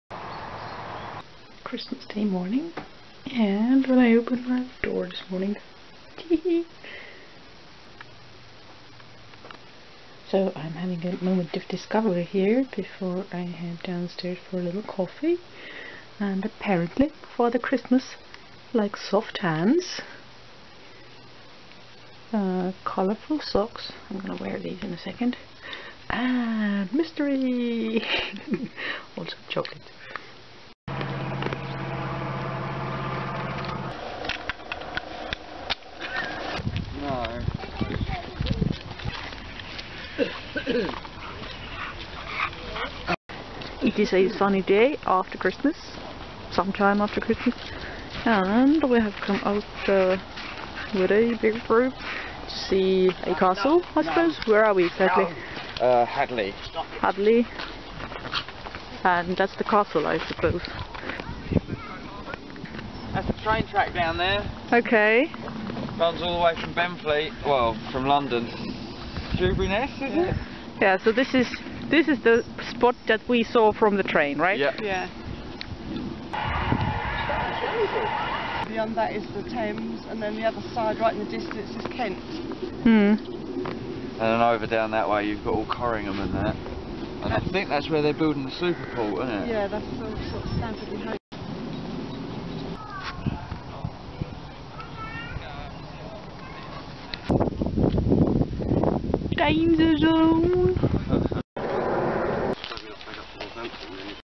Essex expedition logs (5). Local customs and local castles.
I make a Christmas Morning discovery, and join the family on a walk to a castle.